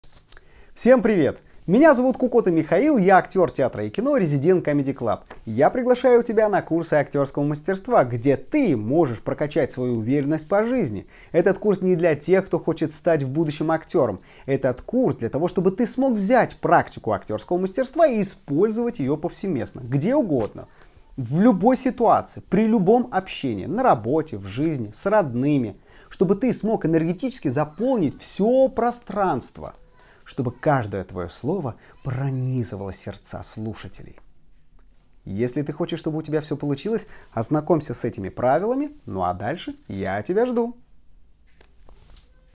Михаил Кукота | Приглашение на курс Актерского Мастерства | Stand-up Школа Игоря Меерсона.
Stand-up School by Igor Meerson представляет:
Авторский курс Актерского Мастерства от Михаила Кукоты.

Михаил Кукота: актер театра и кино, резидент Comedy Club.
Stand-up Школа Игоря Меерсона: stand-up комедии, ораторское искусство, сценическая речь, драматургия, импровизация, актерское мастерство, эффективная коммуникация в жизни и бизнесе.